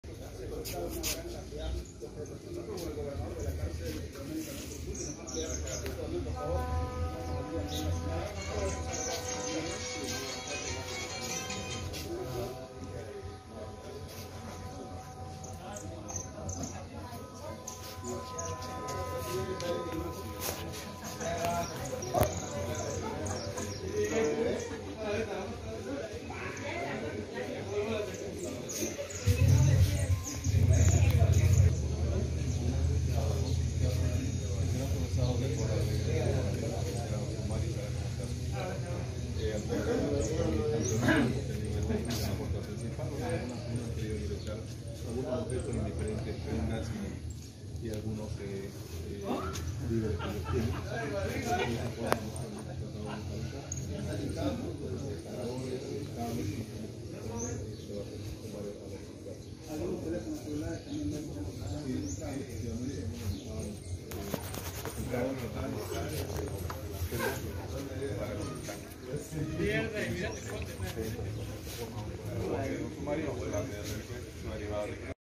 REQUISA SORPRESA EN LA CARCEL DE PALMASOLA, DOCE PRIVADOS DE LIBERTAD SON AISLADOS.
Producto del operativo se encontraron una gran cantidad de armas blancas, celulares, cargadores, somníferos, alcohol y licores destilados de forma casera.